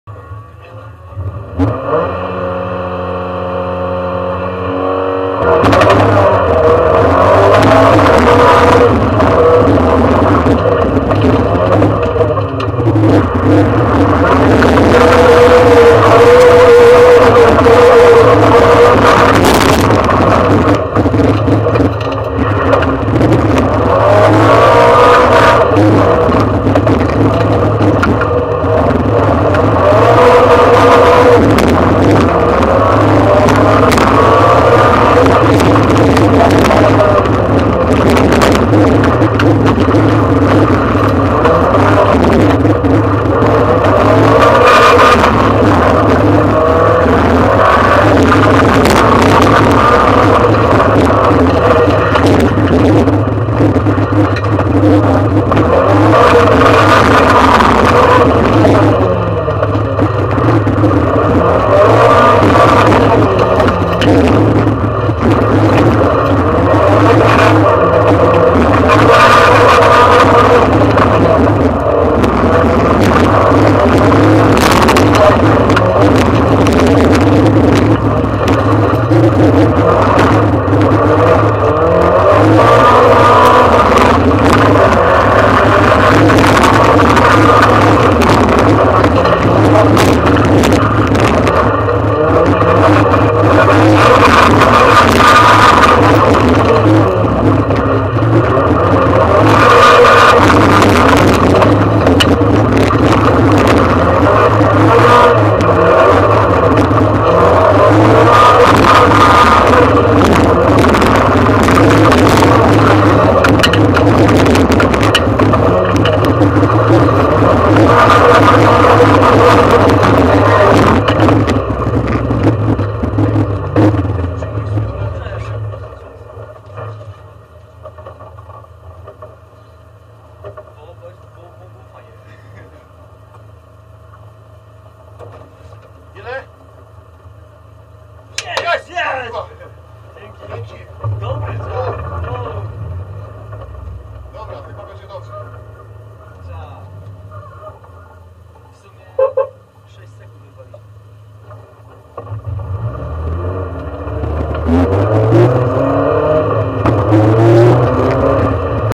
KJS Lądek dla Kibiców
Nasz najlepszy przejazd z czasem 1:54,52. Zajęliśmy 3. miejsce w klasie i 5. w generalce :)